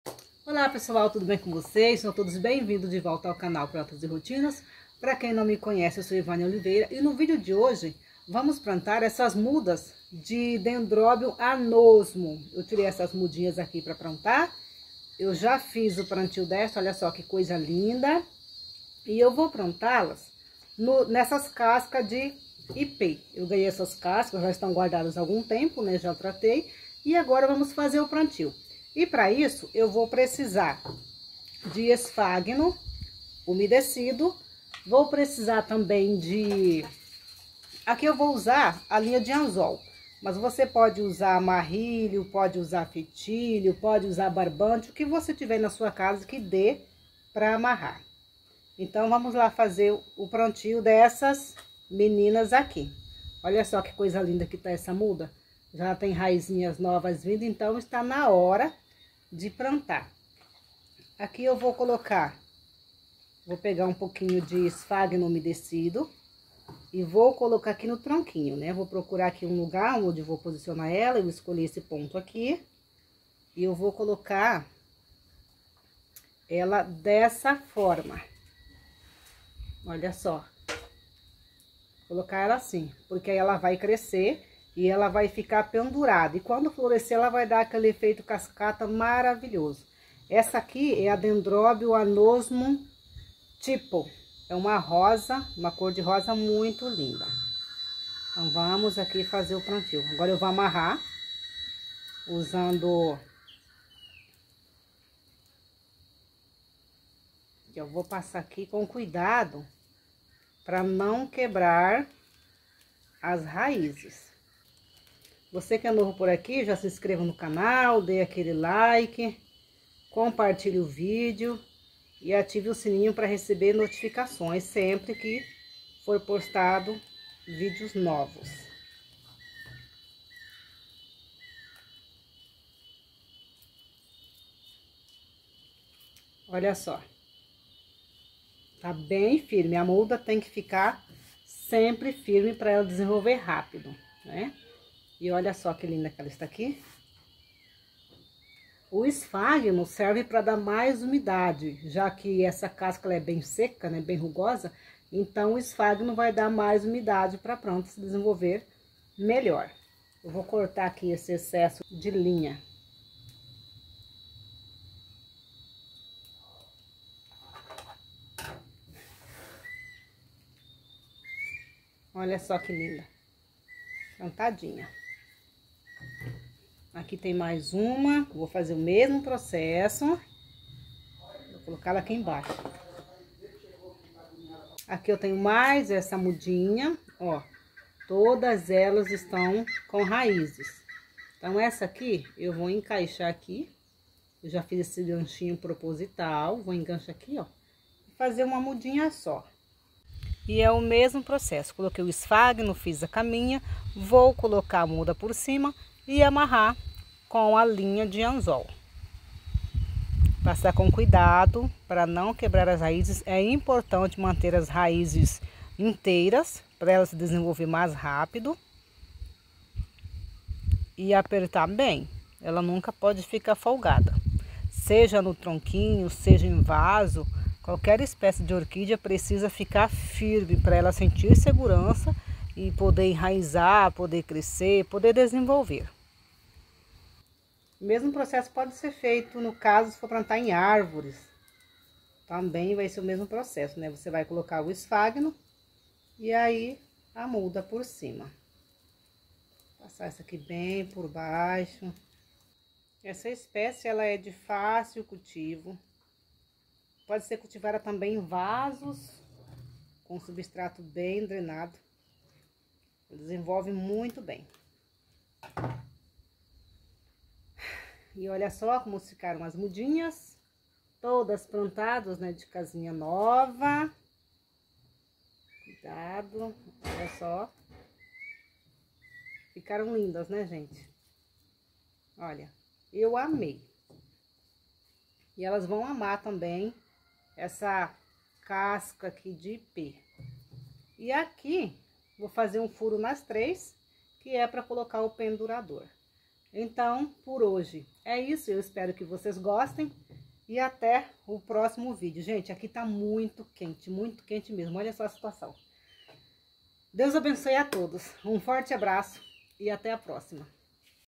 PLANTEI DENDROBIUM ANOSMUM NA CASCA DE IPE
Plantei Dendrobium Anosmum na Casca de Ipe#orquideas